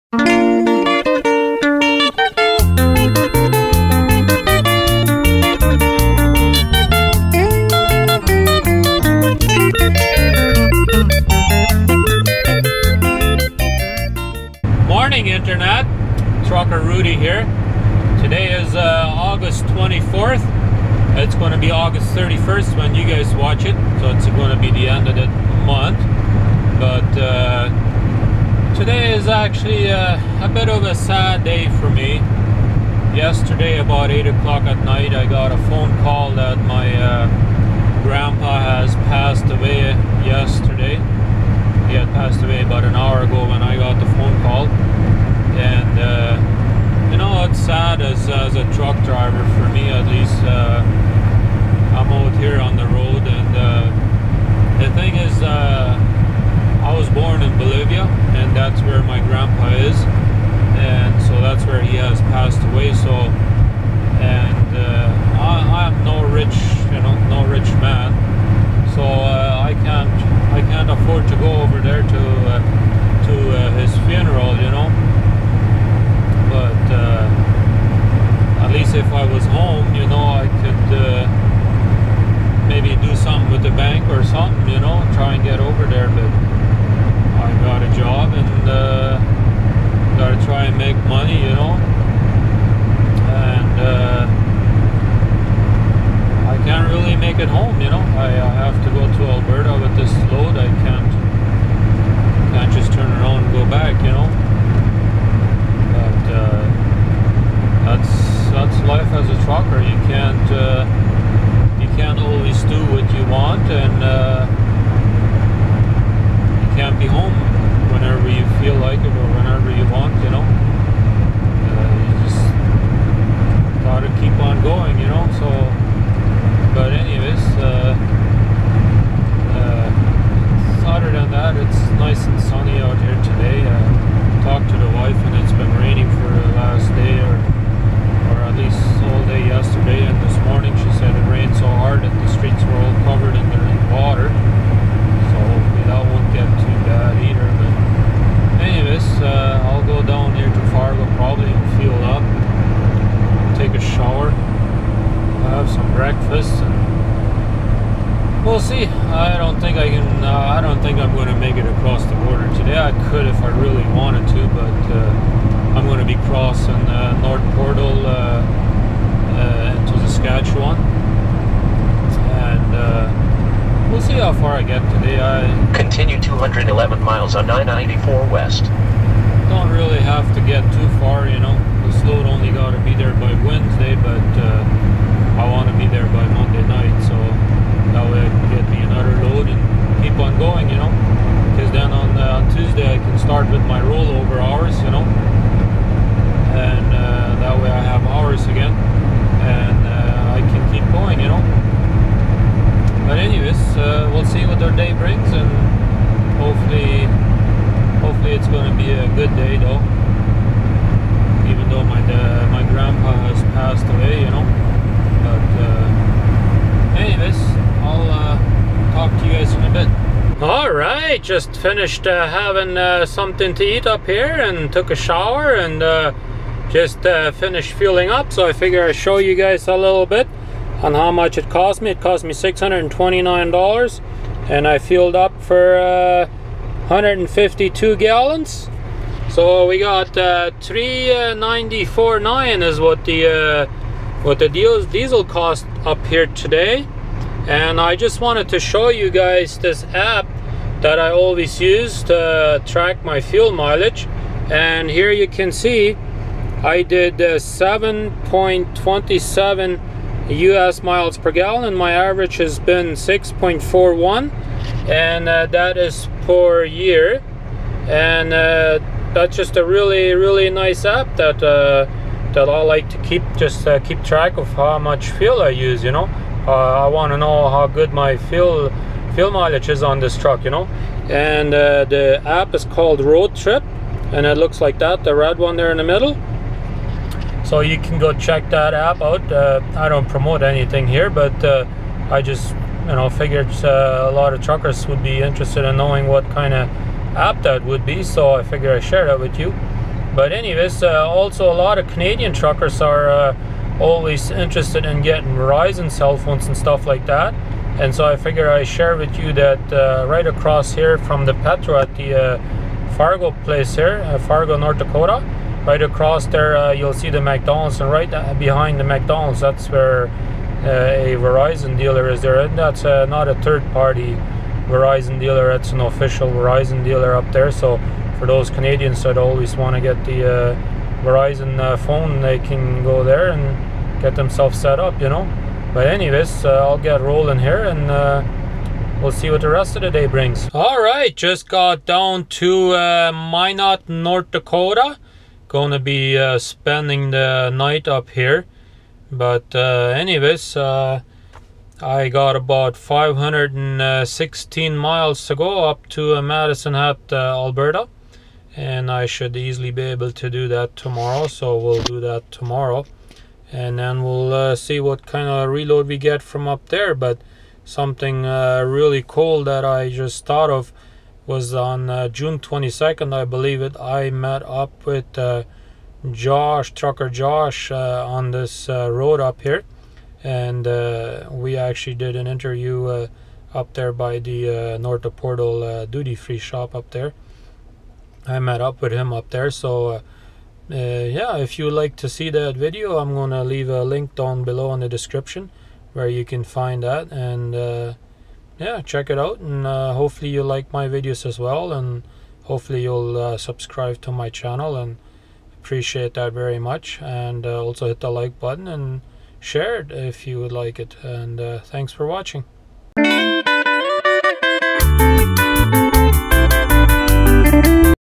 My Trucker life on the road 08.24.14 North Dakota I met trucker Josh
it's a sad day for me because my grandpa died.
Going through North Dakota this is where I met trucker Josh

I'm Rudi, Man from Bolivia. and I live in Manitoba I document and make videos (vlog)
on my daily life travelling across North America.